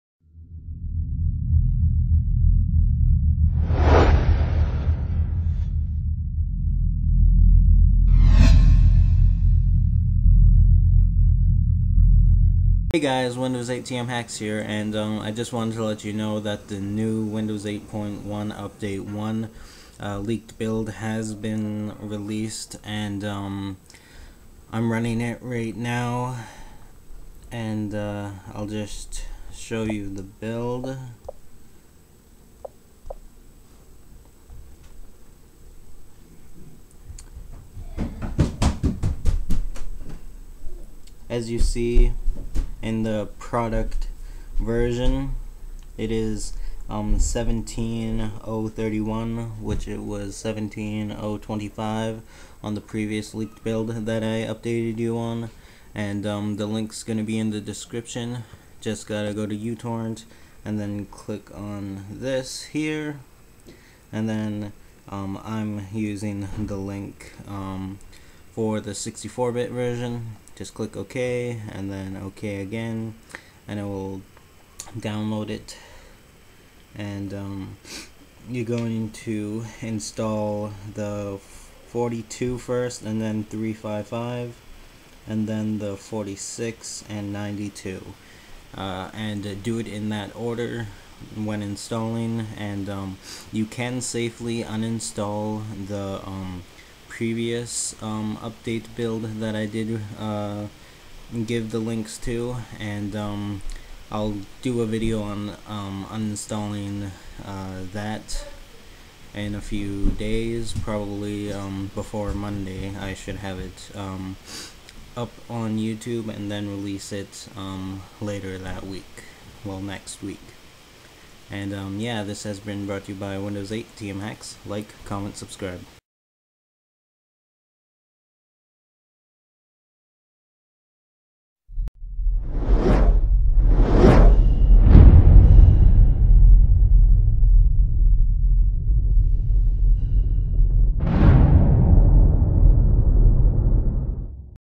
Windows 8.1 Update 1 Leaked! (Build 9600.17031)
Hey guys!

A new update to be released soon has been leaked!

NOTE! Delete the "-" from them to work properly with uTorrent:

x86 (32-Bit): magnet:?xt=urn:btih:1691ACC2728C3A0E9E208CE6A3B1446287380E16

x64 (64-Bit): magnet:?xt=urn:btih:807C749A651D15E92C824FD26D946151E208598B